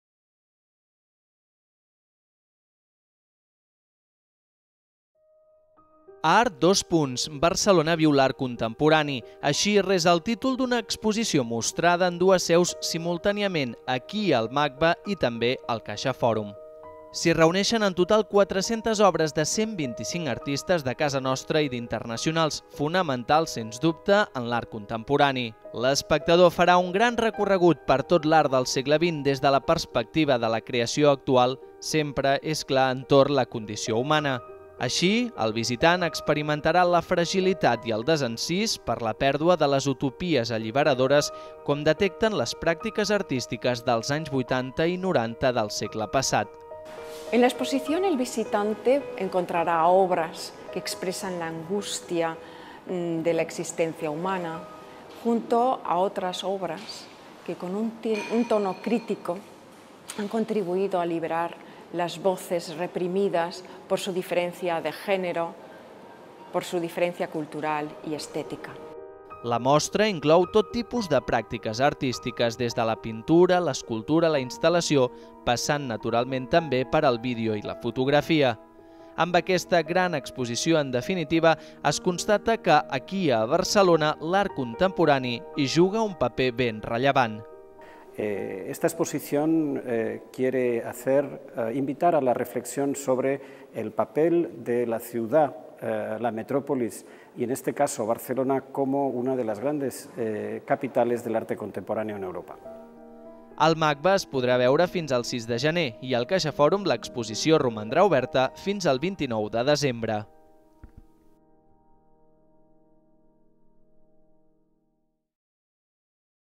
Barcelona viu l'art contemporani
En l'exposició "Art, dos punts", l'espectador experimenta la fragilitat i el desencís per la pèrdua de les utopies alliberadores, com així detecten les pràctiques artístiques dels anys vuitanta i noranta.